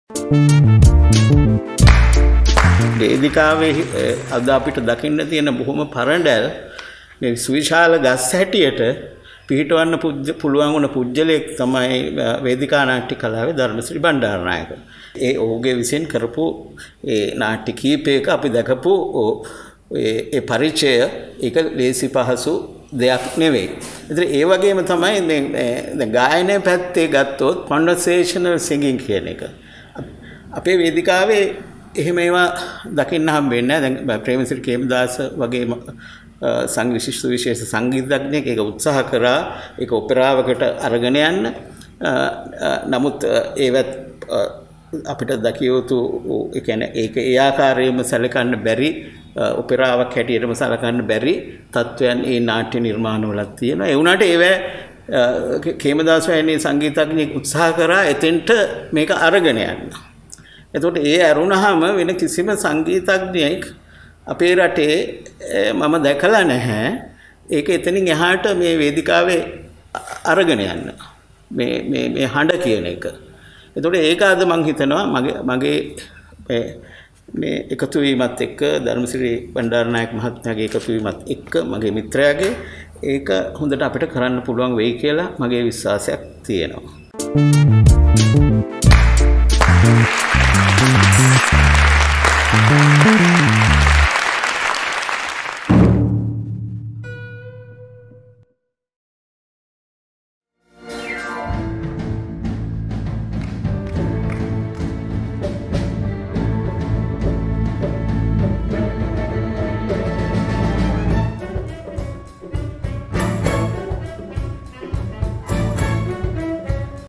Theatrical - Conversational Singing (සංවාදාත්මක ගීත ගායනය) ලංකාවේ විශ්ව විද්‍යාල වල උගන්නනවද?
TalentAsia නාට්‍ය හා රංග කලා පදනම් ඩිප්ලෝමා පාඨමාලාව ගැන Dharmasiri Bandaranayake සහ Kithsirimevan Jayasena සමග Niroshan Illeperuma මොරටුව TalentAsia රංග භූමියේ දී සිදු කළ සංවාදය සහ පූර්ව-දර්ශනය. පාඨමාලාවට ලියාපදිංචි වීමේ අවසාන දිනය නොවැම්බර් 30. ලින්ක් එකෙන් අදම ලියාපදිංචි වන්න!
Talent Asia Team - Vasantha Wakkumbura, Viranga Sandeepa, Duminda De Silva, Srinath De Silva, Peshala Serasinghe, Yashodha Wijesinghe, Thakshila Lankathilaka, Damith Lahiru, Panduka Dharshana, Hashini Herath, and Shadeesha Kumarasiri